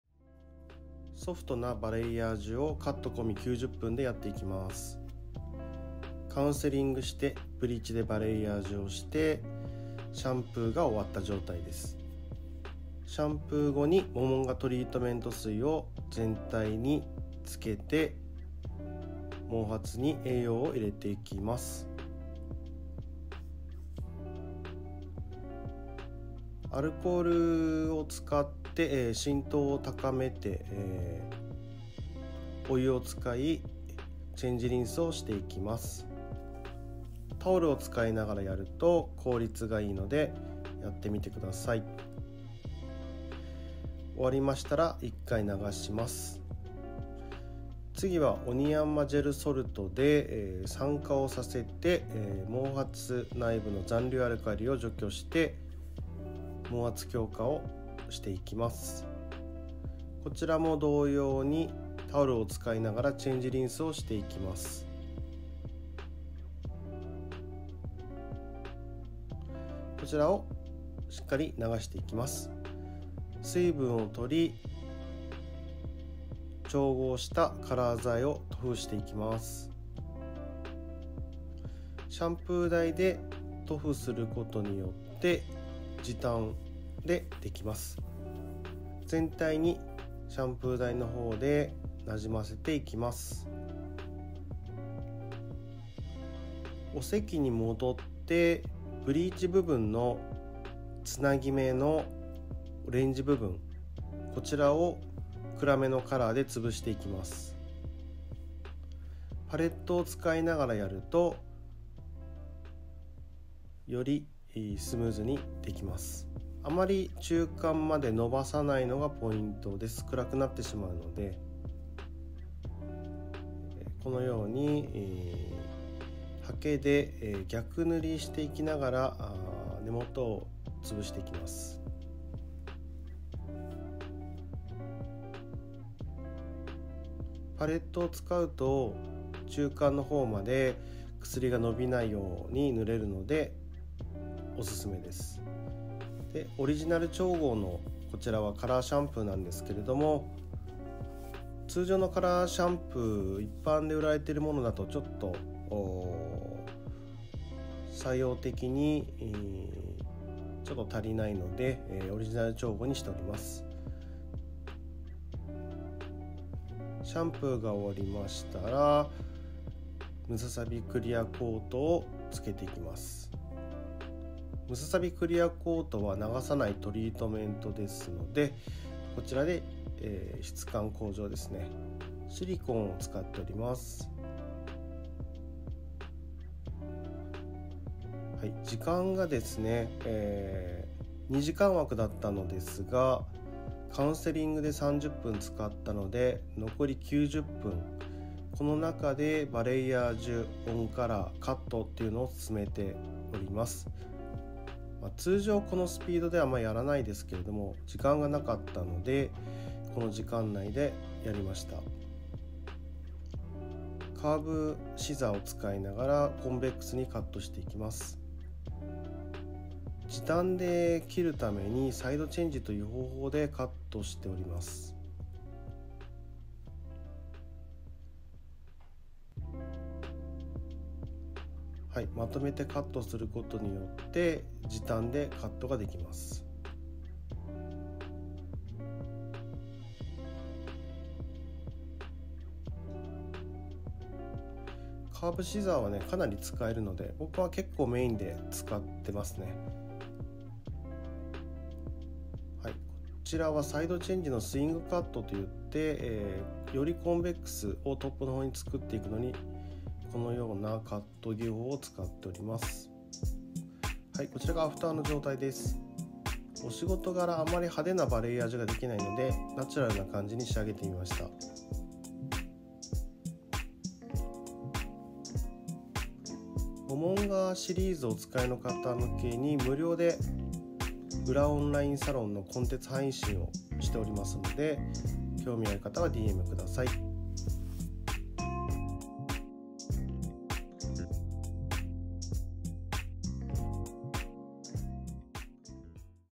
【最速神技】90分でバレイヤージュ＋オンカラー＋ヘアカットをしてみた！ケアブリーチのトリートメント方法とやり方を公開
★動画で使用したトリートメントの詳しい情報はこちらから